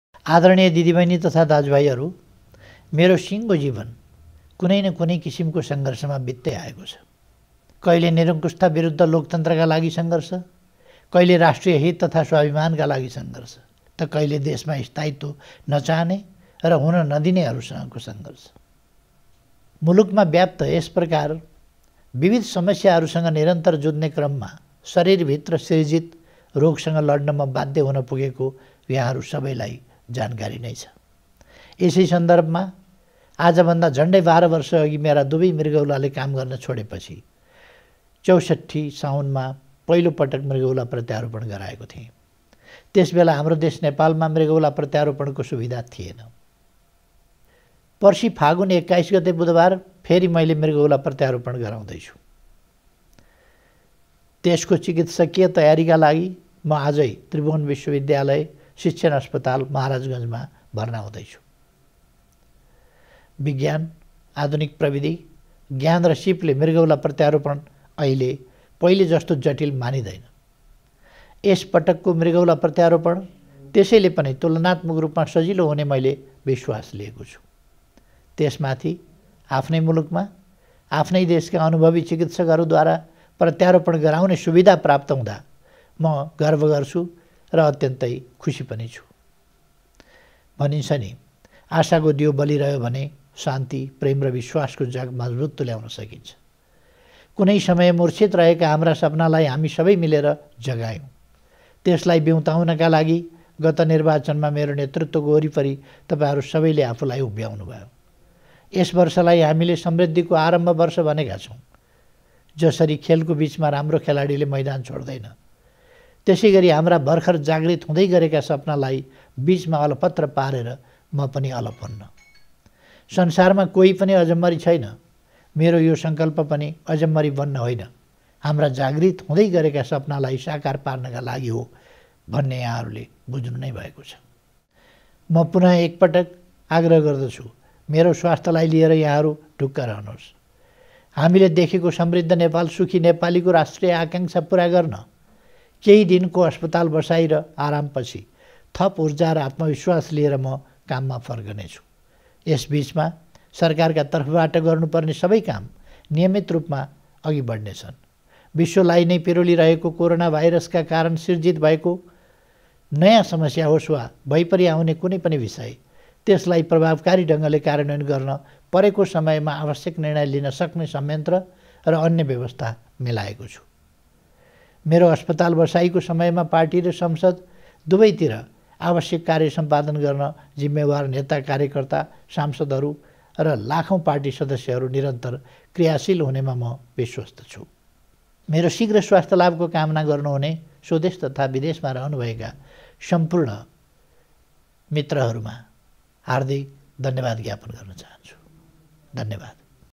मृगाैला प्रत्याराेपण गर्न जानुअघि प्रधानमन्त्री ओलीले भावुक हुँदै दिए यस्ताे सन्देश | PM KP OLI
CONNECT WITH BAZARMANDU